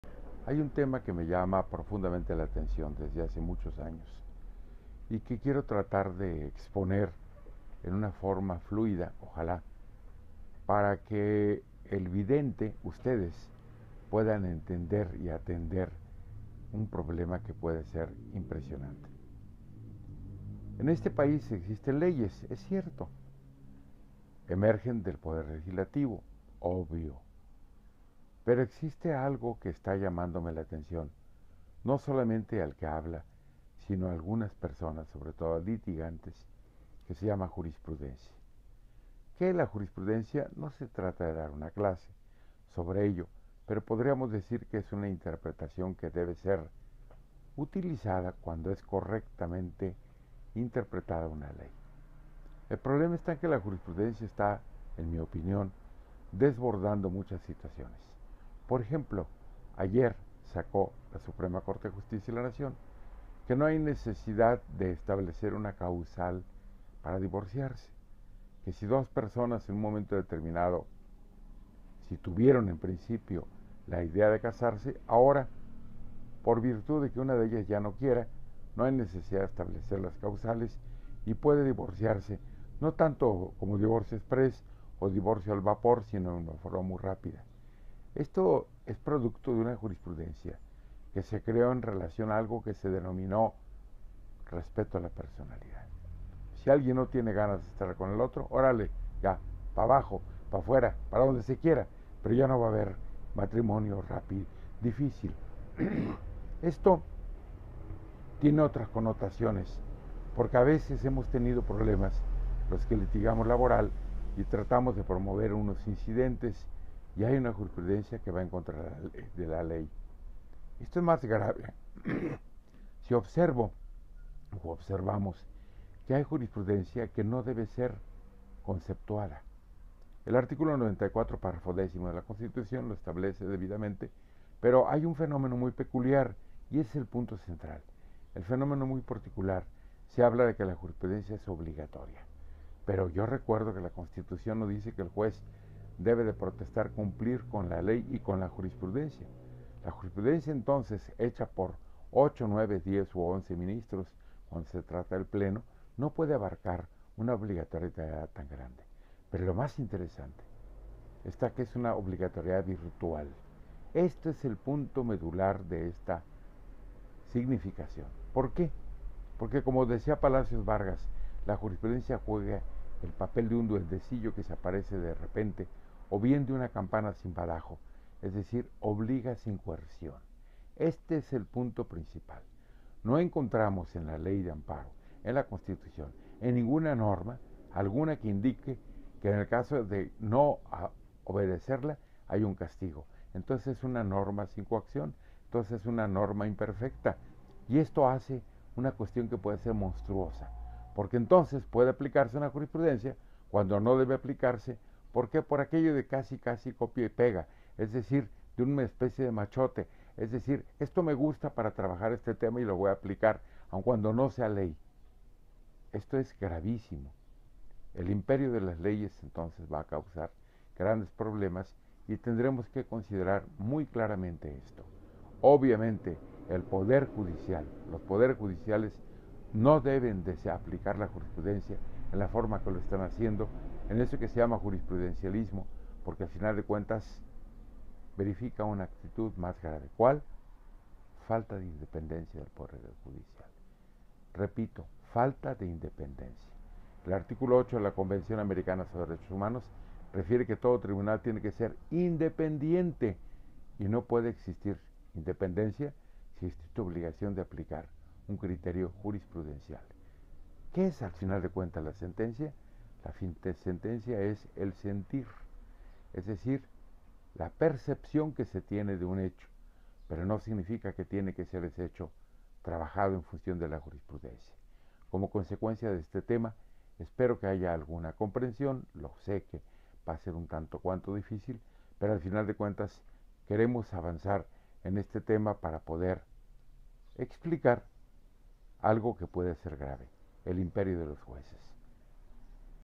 Vale Saber/// José Carlos Guerra Aguilera , la Jurisprudencia no es norma
En esta entrega el abogado, notario y estudioso -con 13 libros en su haber- se enfoca a la Jurisprudencia y sus interpretaciones, aunque aclara: no es norma.